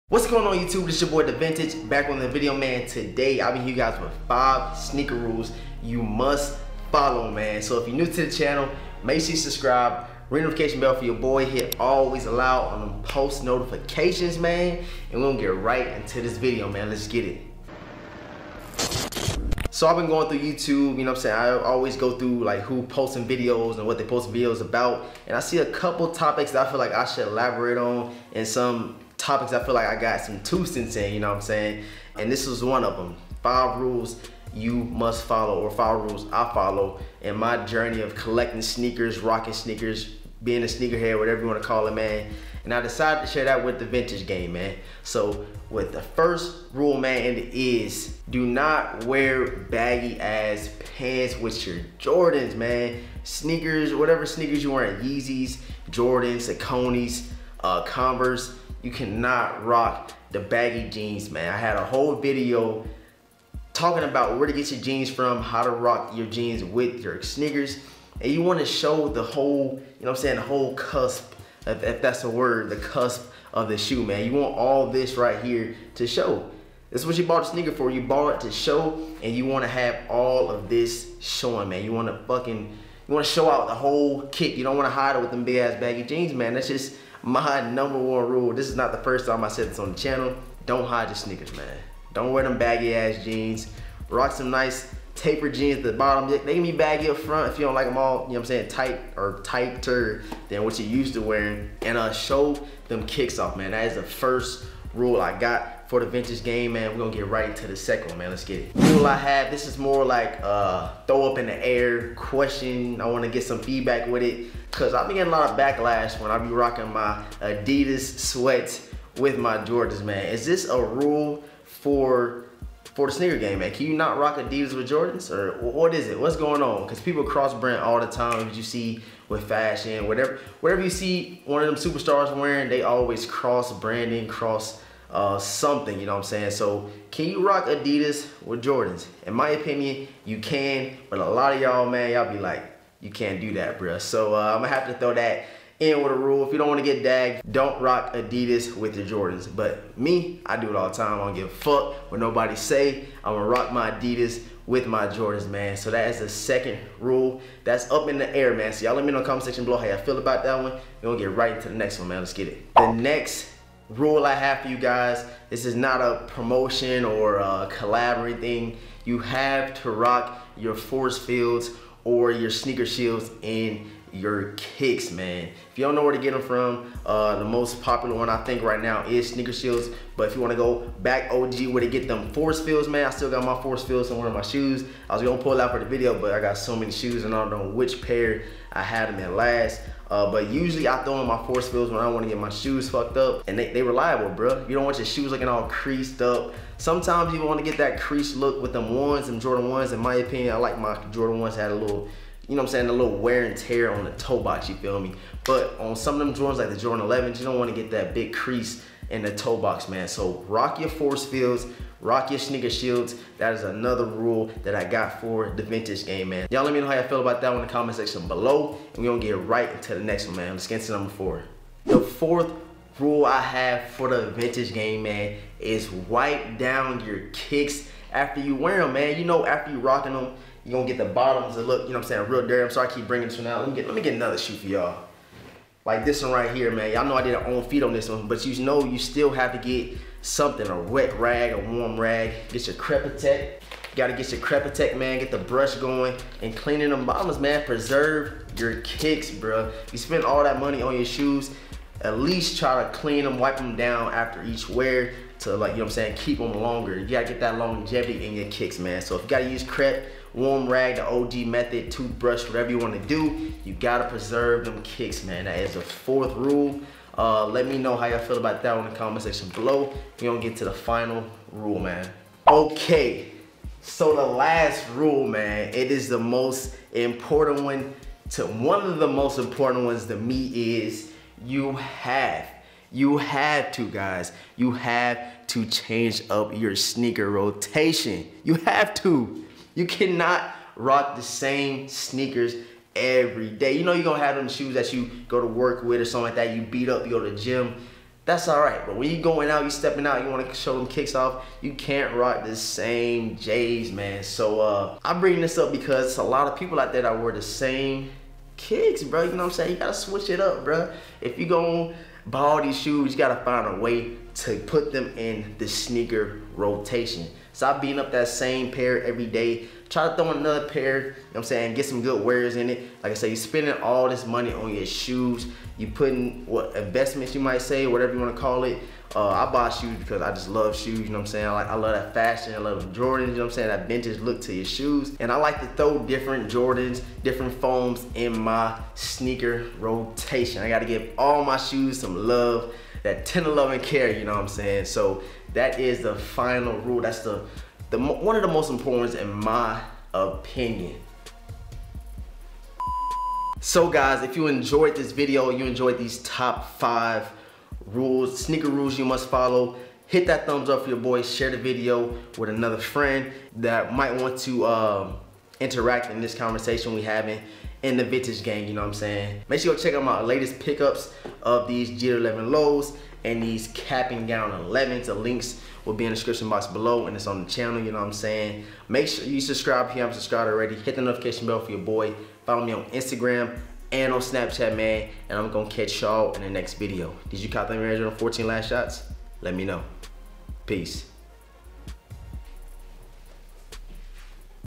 5 SNEAKER RULES YOU MUST FOLLOW!? (2018)
What’s going on Vintage Gang! Today I’ve been watching some videos on youtube and I wanted to give my two cents in some of these topics. The first one being, 5 Sneaker Rules You Must Follow! Let me know if I am missing some rules, also let me know how you feel about the Adidas with Jordan cross brand. Catch you guys in the next video!

the.gook